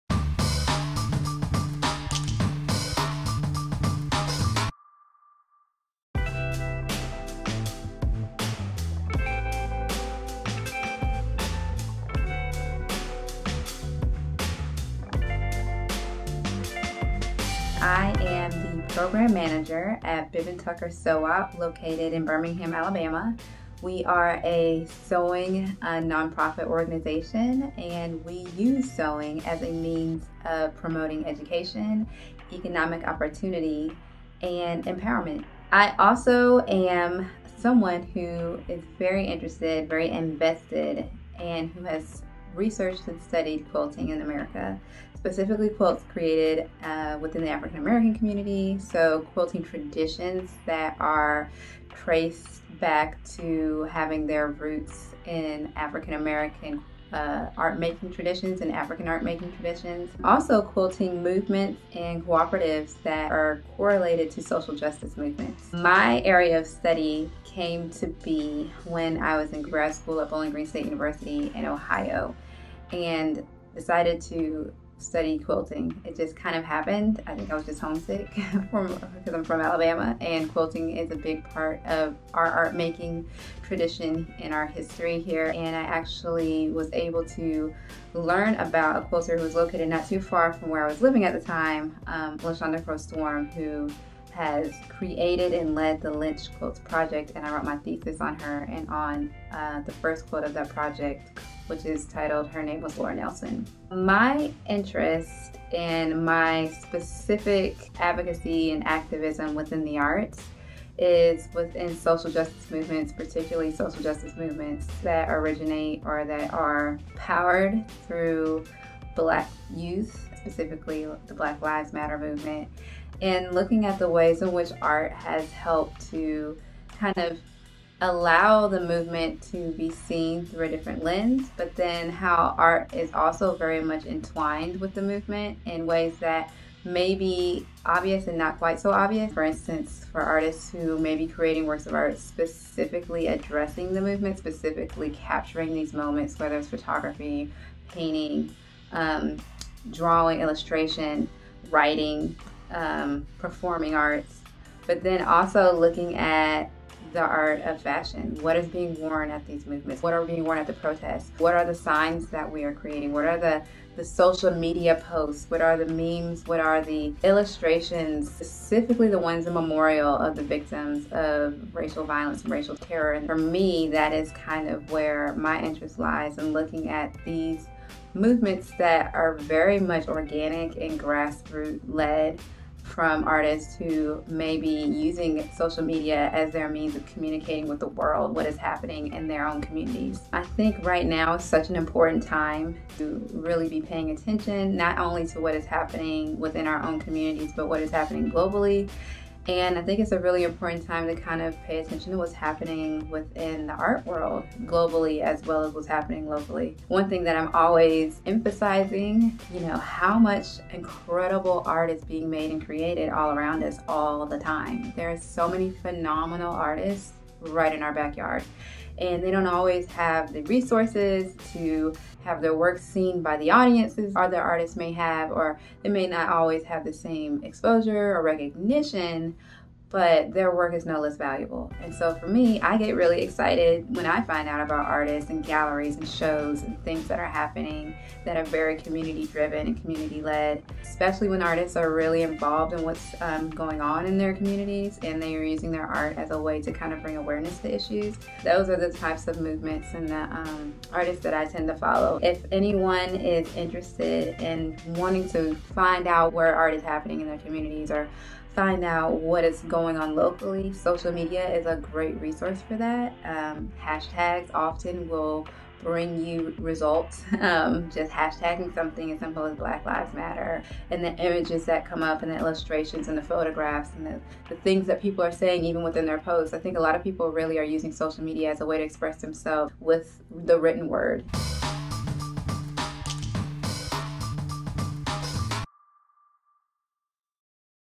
Viola Ratcliffe on the intersection of art and activism | Inspiring Inclusion in the Arts
Viola Ratcliffe is a Birmingham, Alabama-based art historian and Program Manager of Bib & Tucker Sew-Op, a hub for sewing activities that promote empowerment, education, and economic opportunity.

Ratcliffe speaks to the power of local art and community while connecting her longstanding interest in quilting traditions to the social justice-oriented artistic creations of today.

Inspiring Inclusion in the Arts originally streamed November 2, 2020 as part of In Our Own Words, our series that brings issues of social justice center stage through art and conversation.